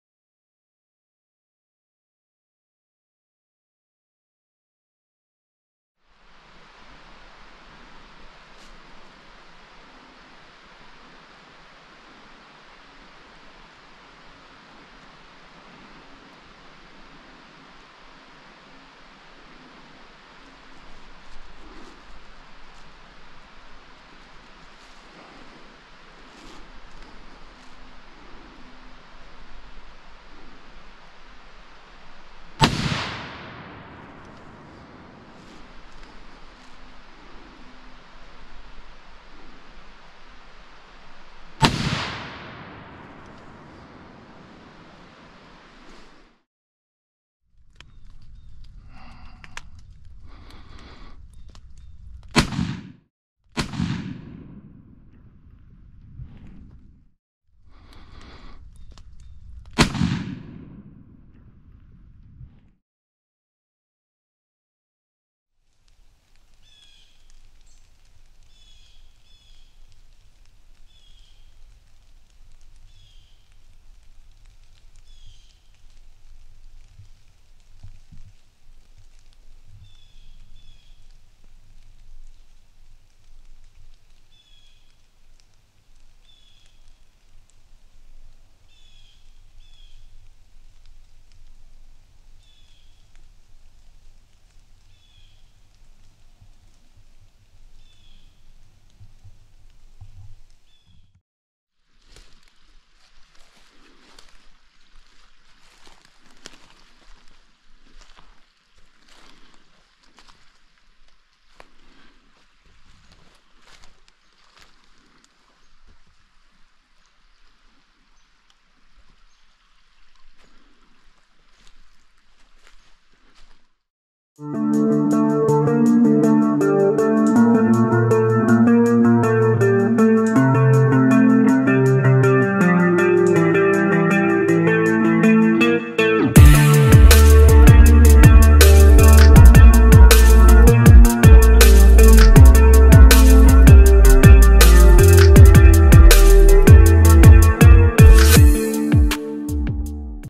Tactacam 6.0 vs GoPro HERO10 Black - A Quick Action Camera Comparison
Tactacam 6.0 vs  GoPro HERO10 Black - A Quick Action Camera Comparison. Trying to figure out what action camera is best for you is not easy. Almost every action camera has strengths and weaknesses. This video shows you in field comparisons and different modes to help you compare these 2 cameras.